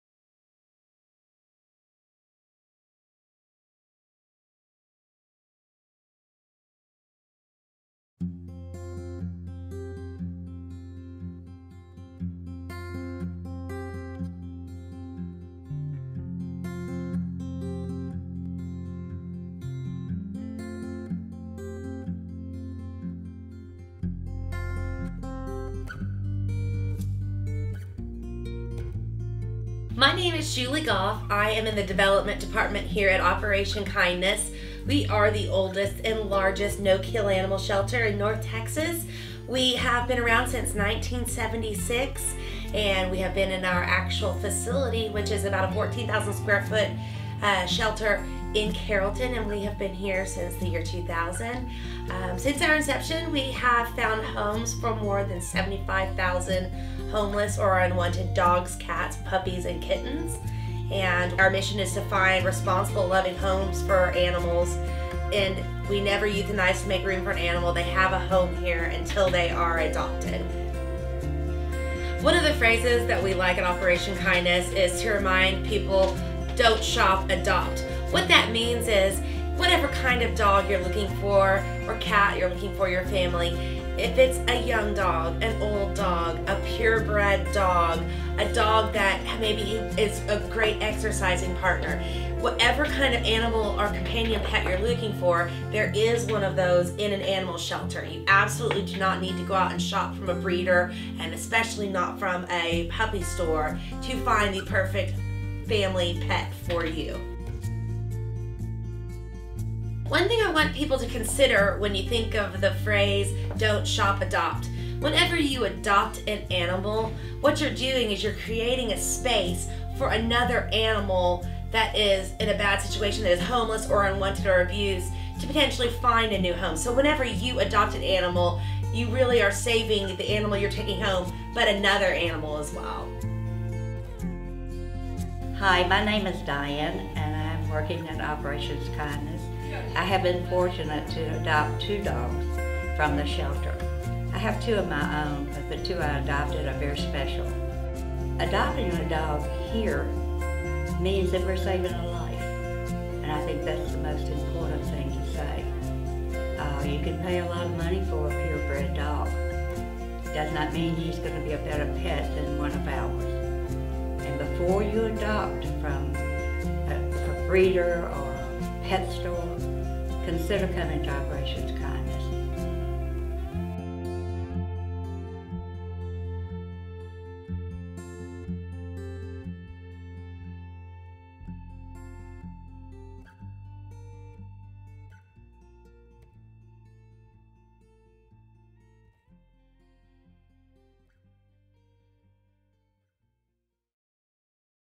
Operation Kindness
Operation Kindness is a nonprofit no-kill animal shelter in Carrollton, Texas. They are North Texas' largest and oldest no-kill animal shelter.